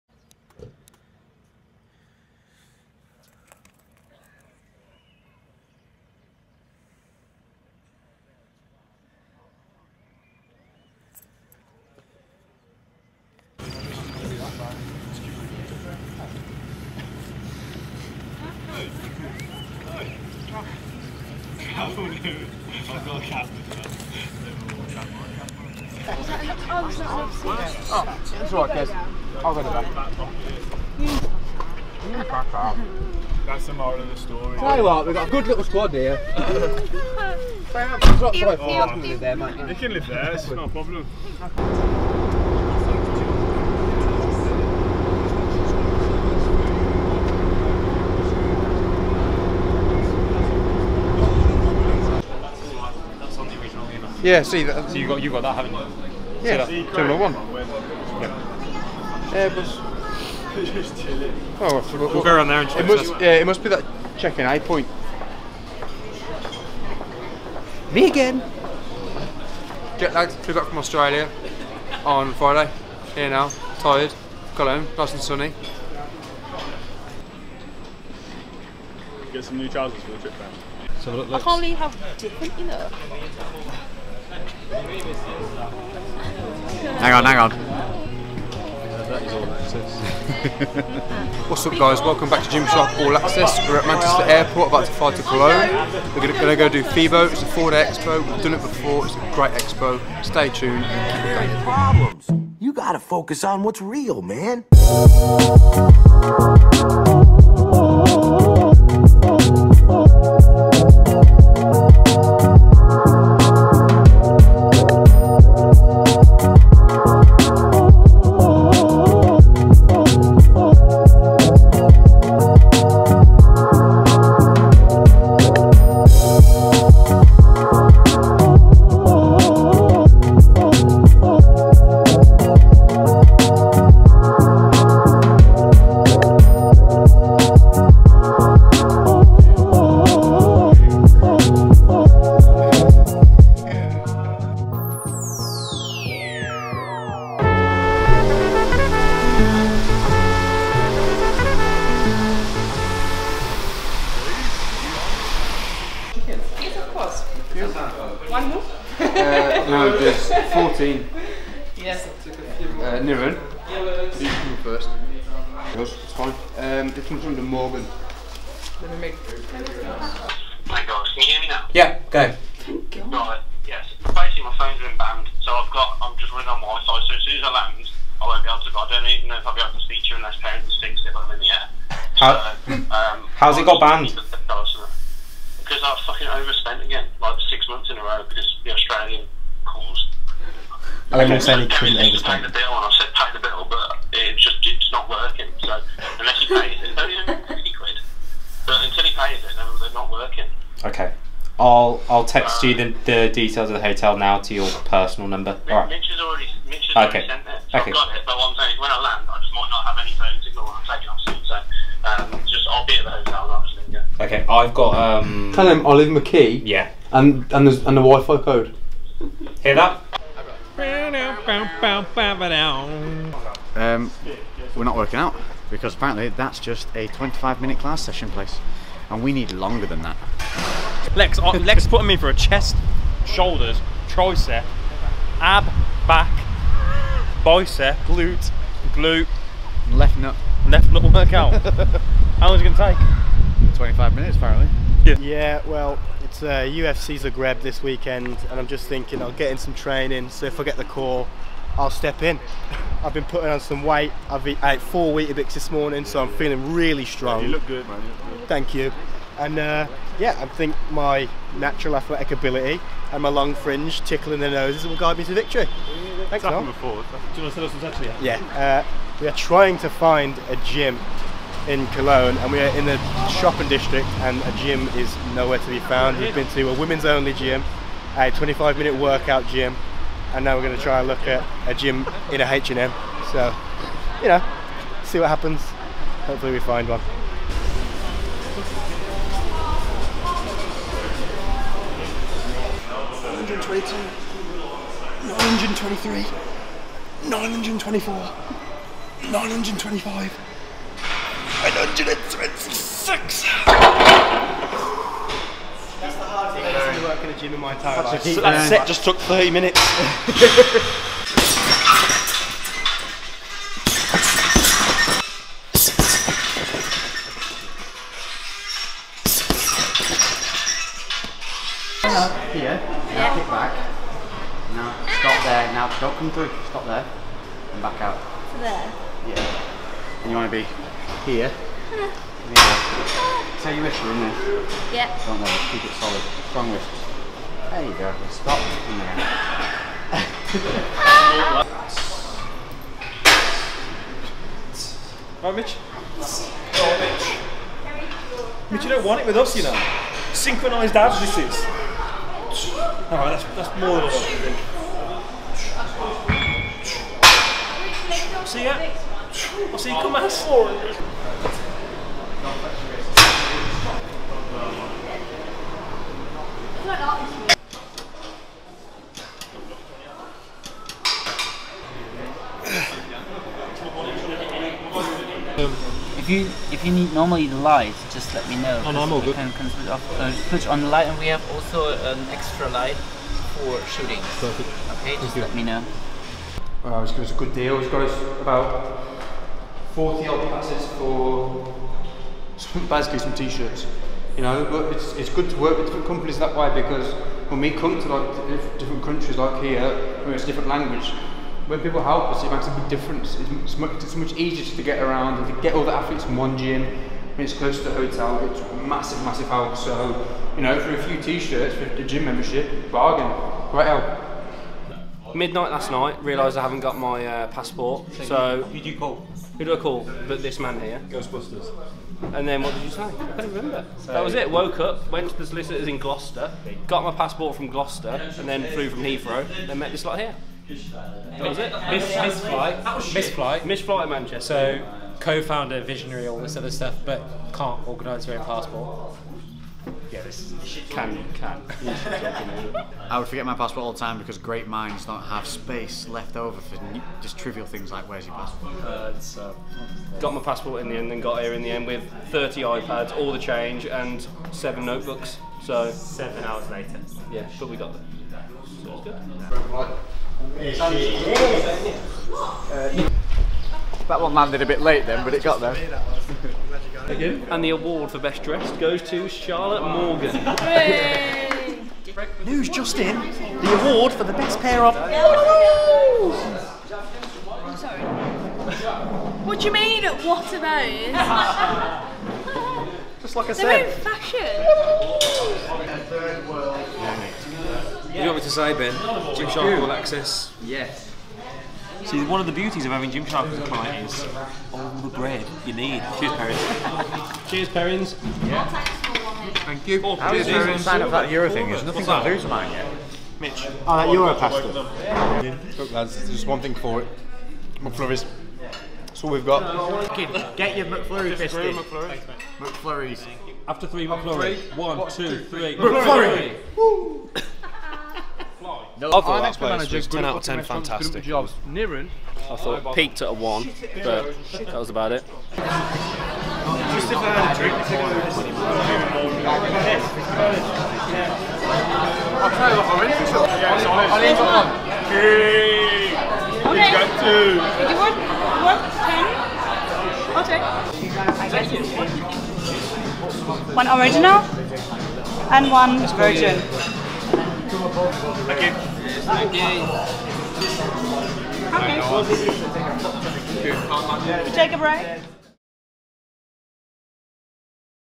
Gymshark All Access - FIBO Cologne, GER. Episode 1 ft. Lex Griffin
Gymshark: All Access shows you how we bring our ideas to life. See every mile travelled and every hour worked by the people who believe in and build our future.